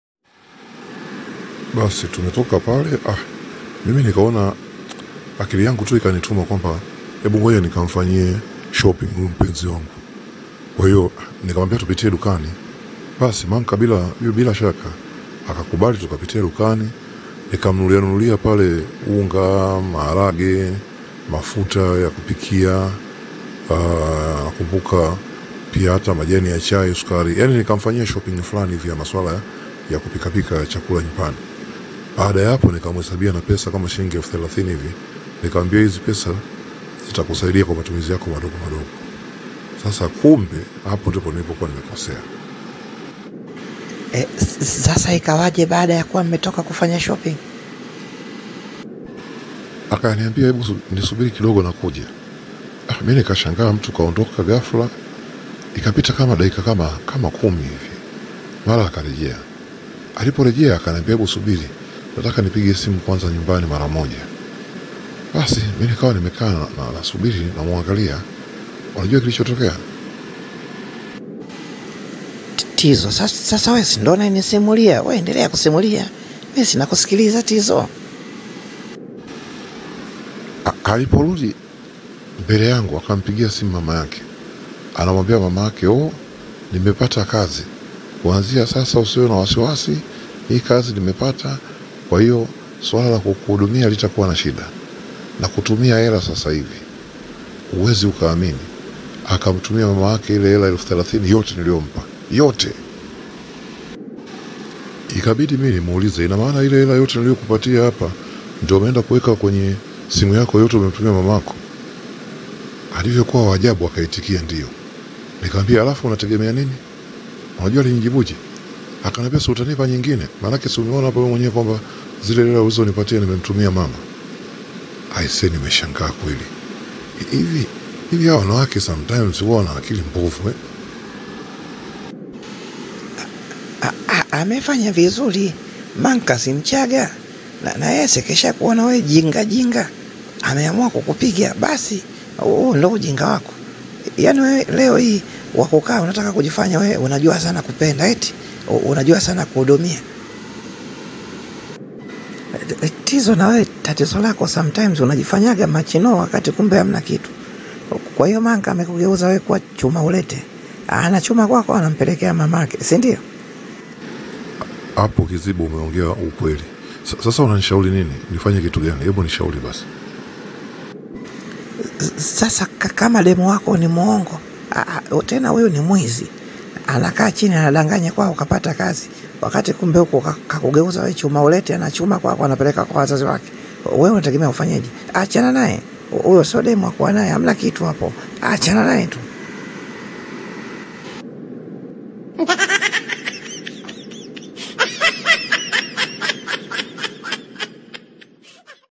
SIUTANIPA HELA NYINGINE
Jamaa amemnunulia vyakula na kumpa hela za matumizi mpenzi wake. Hiyo ilionyesha kuwa anamjali. Mwanamke ana amua kugawa hela kwa mama yake. Anadanganya kapata kazi.